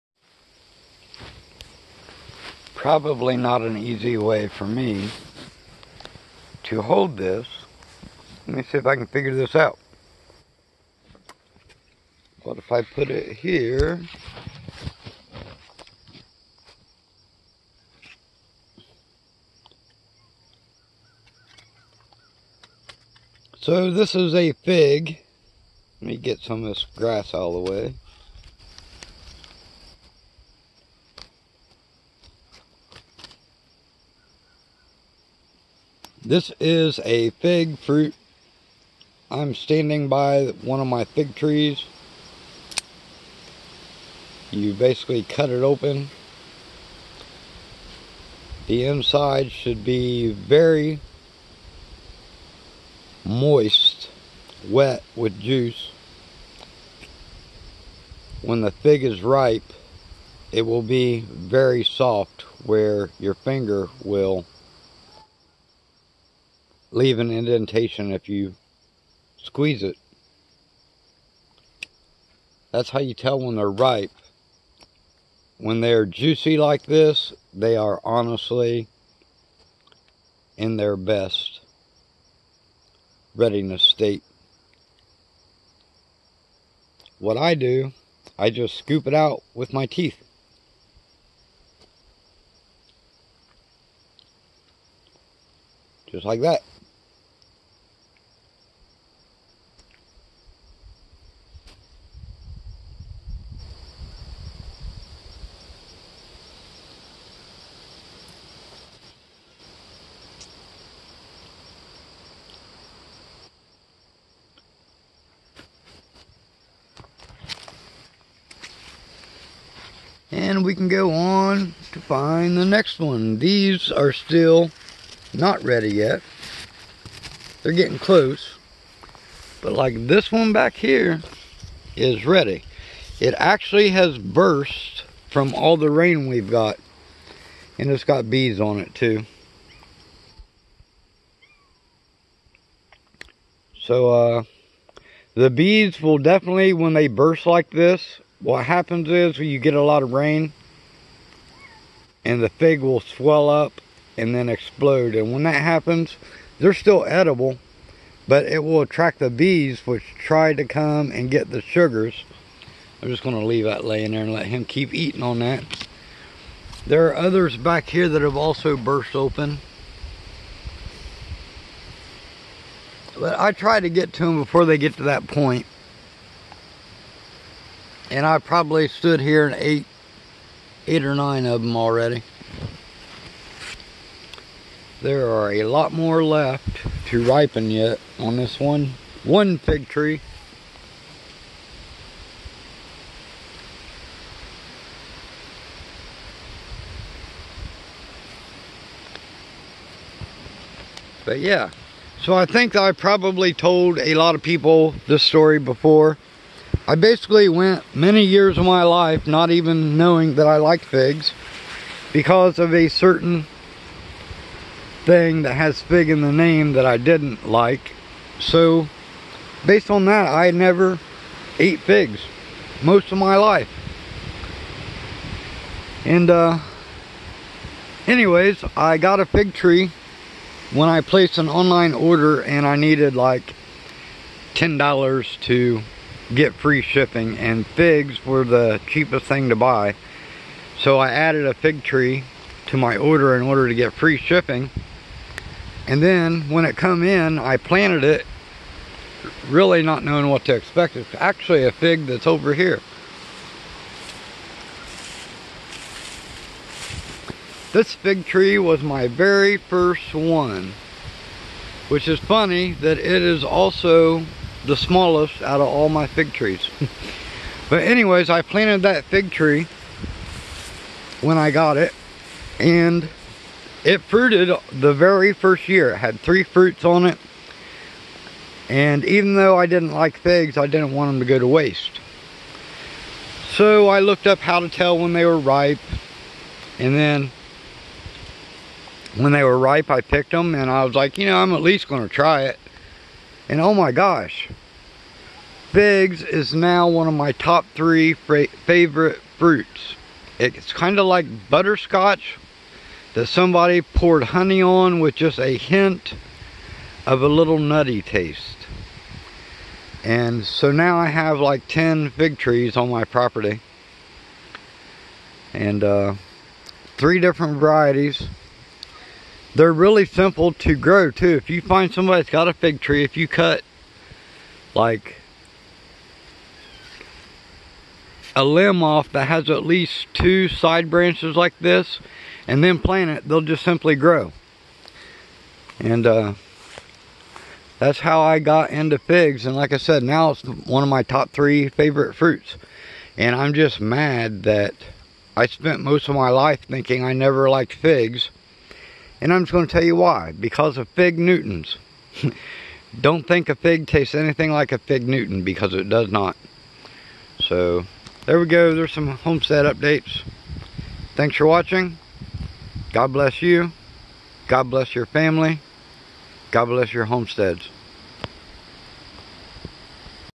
ripe figs on the homestead
if you have followed my channel very long, you have probably heard the story how i grew up hating figs because of fig Newton's. Real fresh figs taste nothing like fig newtons. They taste like someone took a butterscotch candy, poured some honey on it, then sprinkled it with macadamia nuts. Figs became one of my top 3 favorite fruits way to late in my life. Now i own around 10 fig trees on my homestead and im trying to make up for all the figs i never got to eat :)  Try them!  In this video i show you how to tell when they are ripe.